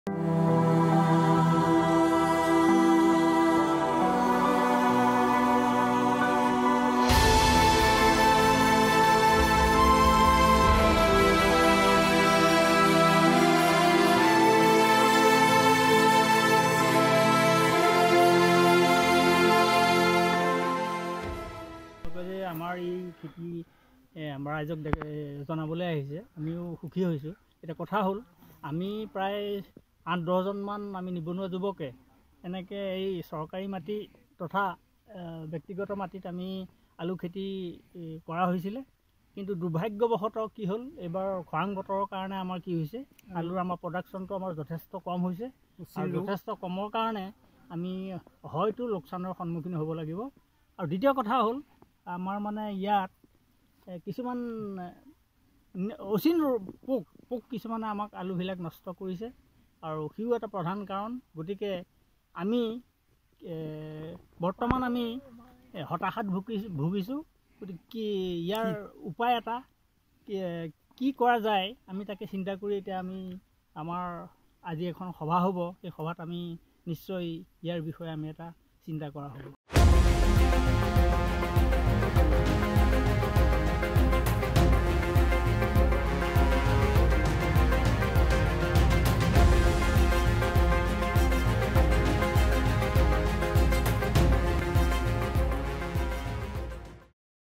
অচিন ৰোগে বিপৰ্যয় কঢ়িয়াই আনিছে আলু খেতিয়কলৈ
▶️অচিন ৰোগে বিপৰ্যয় কঢ়িয়াই আনিছে আলু খেতিয়কলৈ। চতিয়াত শুকাই গৈছে বিঘাই বিঘাই আলু গছ।